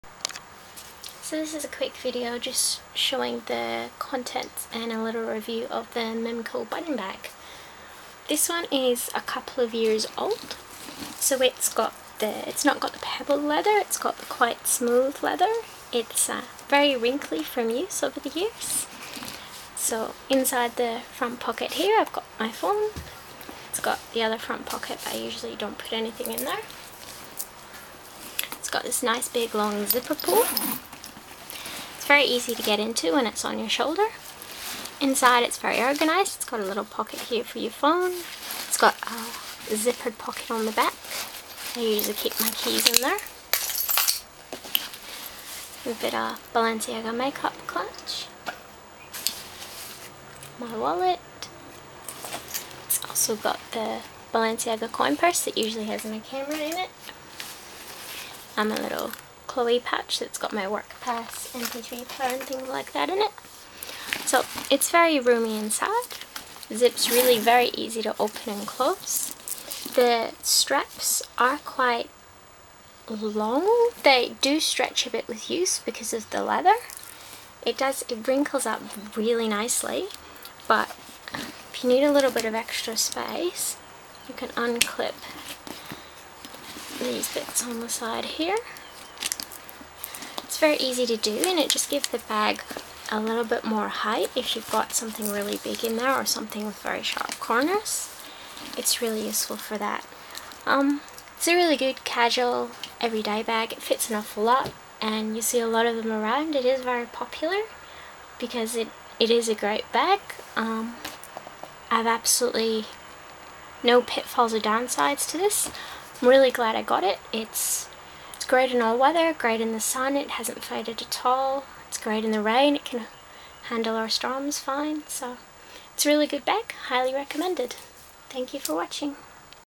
Review and Contents of my Mimco black Button Bag
Short review and contents of my Mimco button bag, in black with black hardware. Purchased in 2007, it's the original button bag style that came out in 2005 with the longer shoulder straps, larger bag, and button pockets.

The button style has been replaced by the turnlock bag now